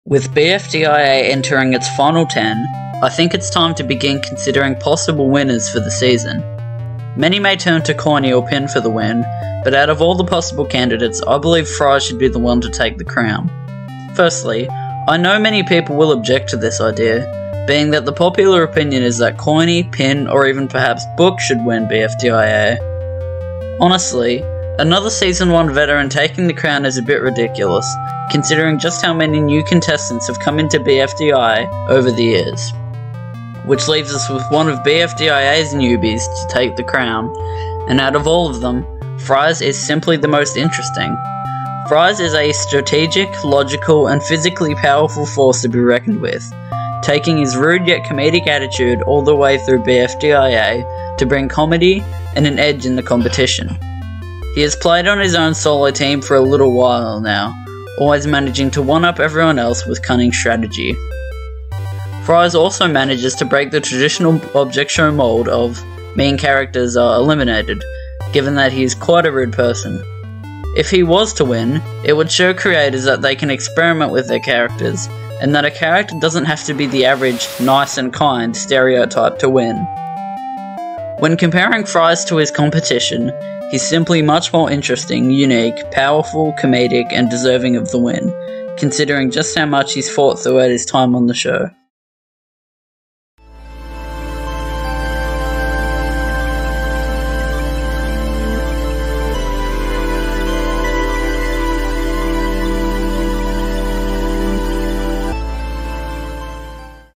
Why Fries Should WIN BFDIA
Many think that a season one veteran like Coiny or Pin should win BFDIA, but the best contender is really Fries, a smart, cunning, strategic, funny and overall entertaining character!